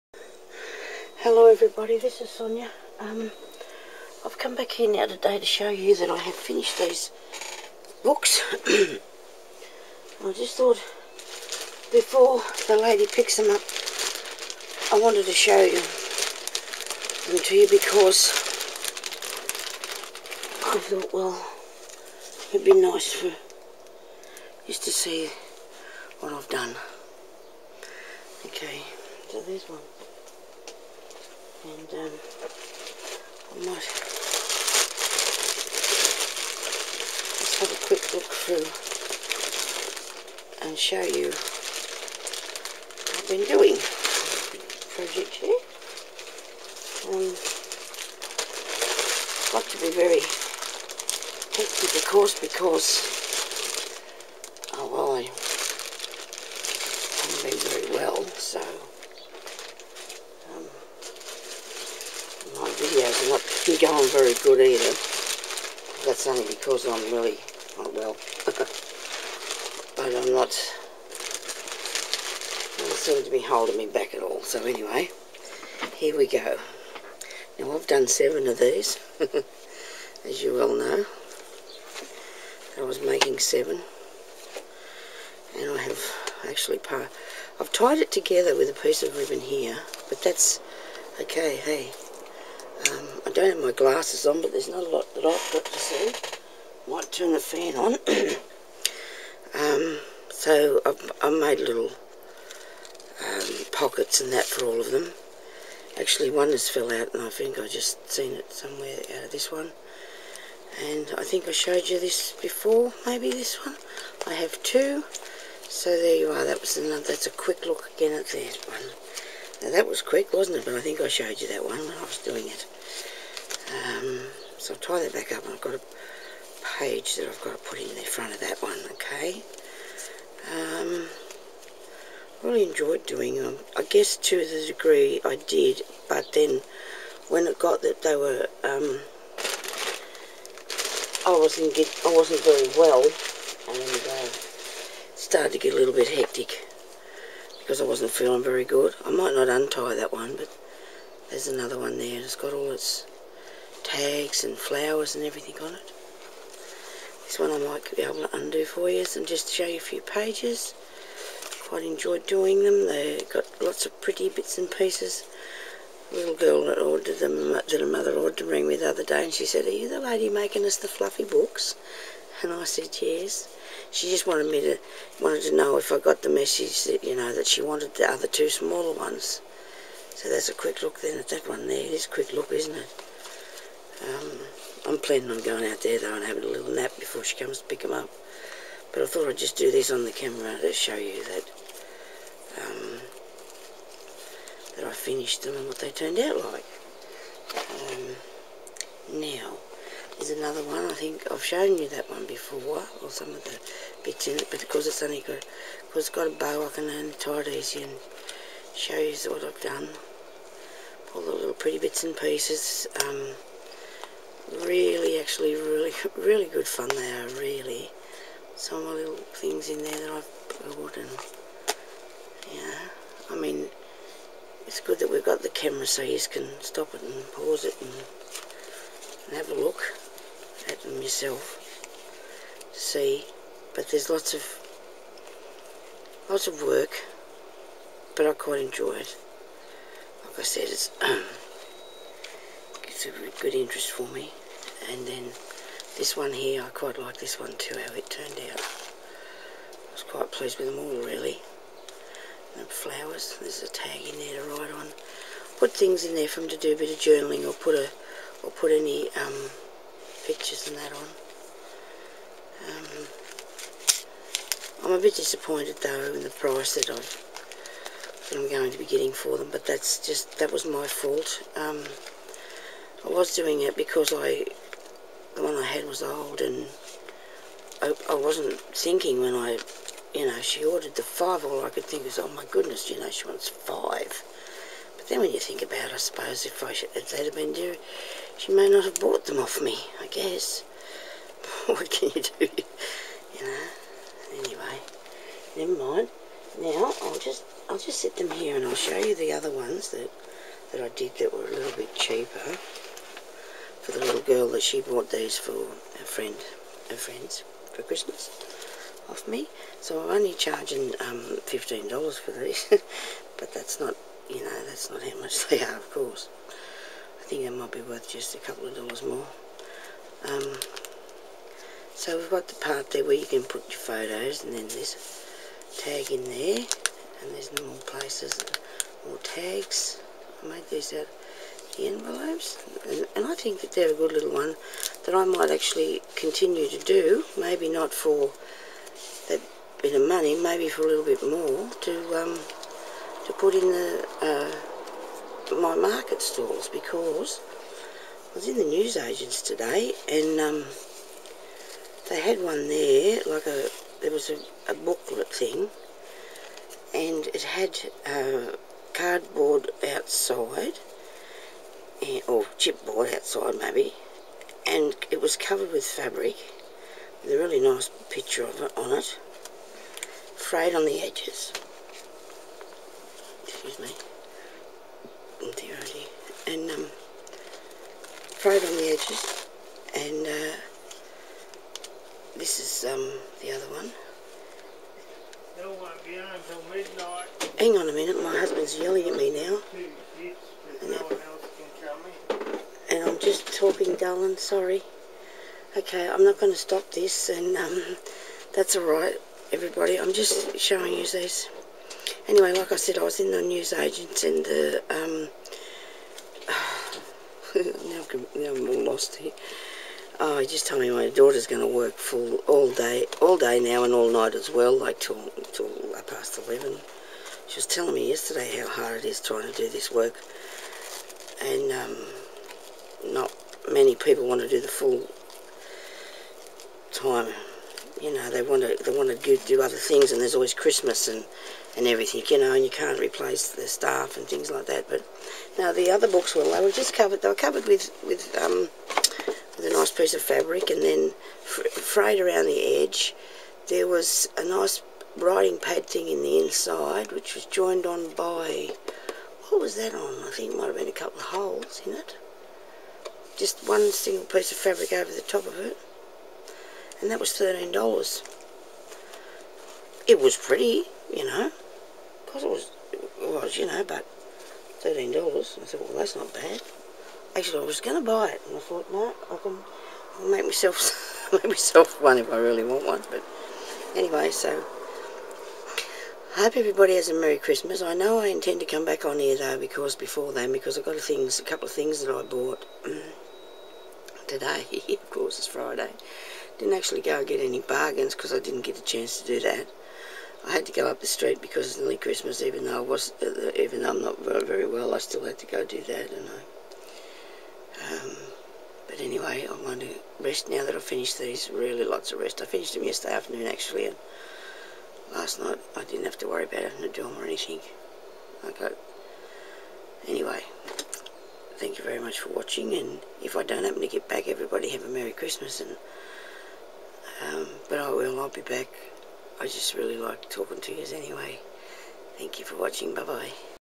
I FINISHED THE ALBUMS
Sorry I have not got in the shot much ! but I think you might get an idea of what I did ! Here is a quick look at the 7 mini albums I made for a customer for her daughters for Christmas